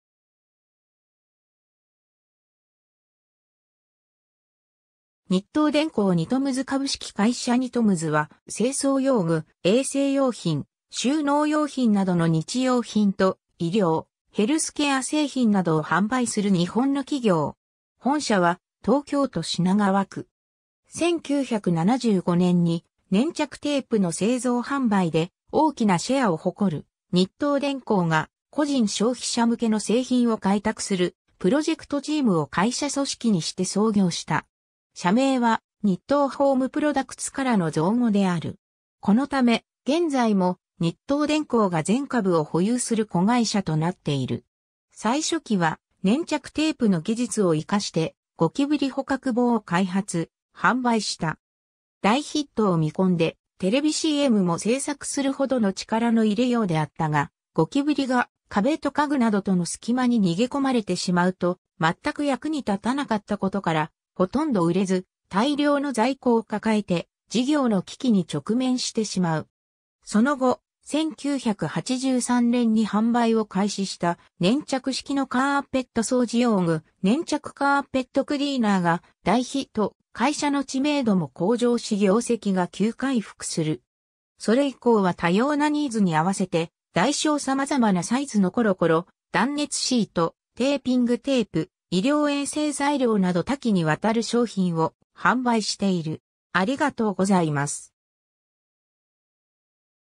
ニトムズ
日東電工 」 ニトムズ 株式会社ニトムズ（英: Nitoms, Inc.）は、清掃用具、衛生用品、収納用品などの日用品と医療・ヘルスケア製品などを販売する日本の企業。
本社は東京都品川区。
 1975年に、粘着テープの製造販売で大きなシェアを誇る日東電工が個人消費者向けの製品を開拓するプロジェクトチームを会社組織にして創業した。
社名はニットーホームプロダクツ（NITTO HOME PRODUCTS）からの造語である。
このため、現在も日東電工が全株を保有する子会社となっている。
 最初期は、粘着テープの技術を活かしてゴキブリ捕獲棒（棒の先に折り畳み式の粘着シートが取り付けられており、上から被せてゴキブリを捕獲したあとに折りたたんでそのままゴミ箱に捨てることで、手を汚さずゴキブリ退治が出来る、というもの）を開発・販売した。
大ヒットを見込んでテレビCMも製作するほどの力の入れようであったが、ゴキブリが壁と家具などとの隙間に逃げ込まれてしまうと全く役に立たなかったことからほとんど売れず、大量の在庫を抱えて事業の危機に直面してしまう。
 その後、1983年に販売を開始した粘着式のカーペット掃除用具「粘着カーペットクリーナー」（現：コロコロ®）が大ヒット、会社の知名度も向上し業績が急回復する。
それ以降は多様なニーズに合わせて大小様々なサイズのコロコロ、断熱シート、テーピングテープ、医療衛生材料など多岐にわたる商品を販売している。